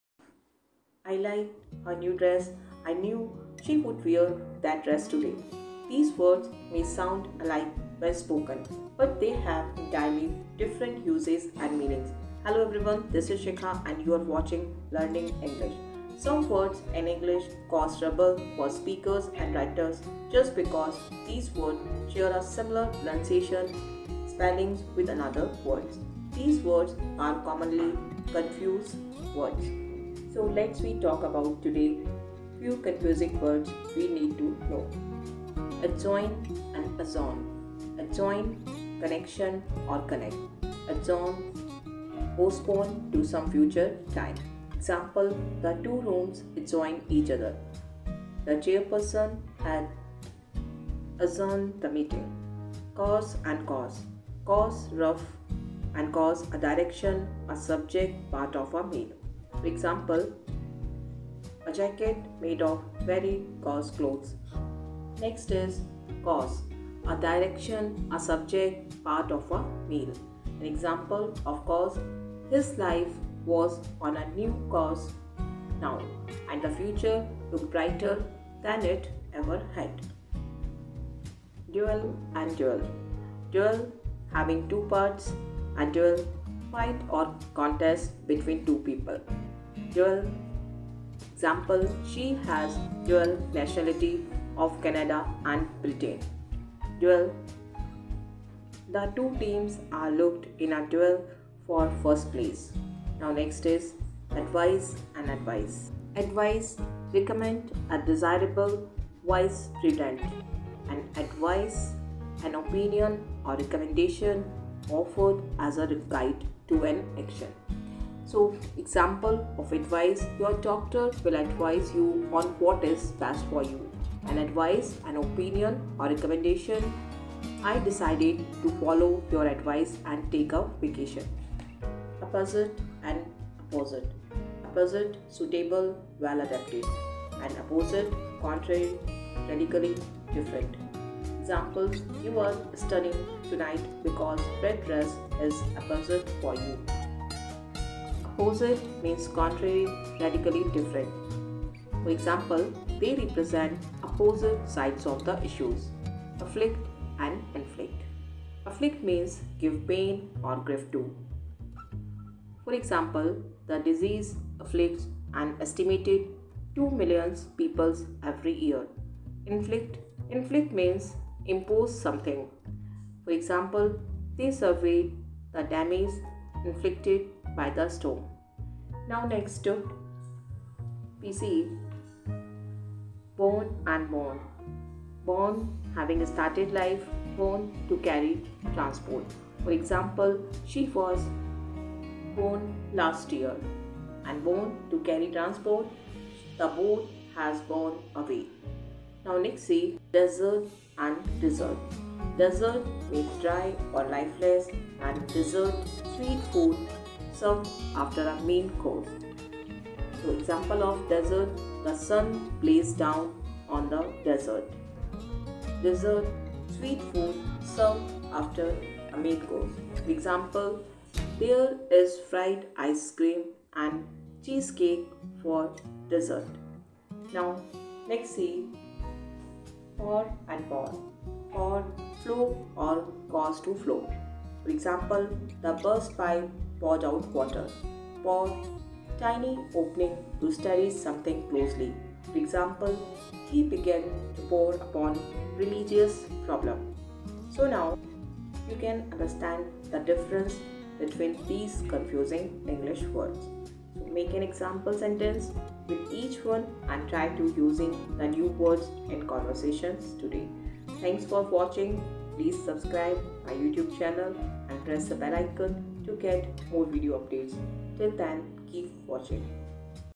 English Confusable words | Learning English | Shikha Mathur
Confused Words that sounds alike but are spelled differently.we will discuss some of the most commonly on confused words with their correct meanings.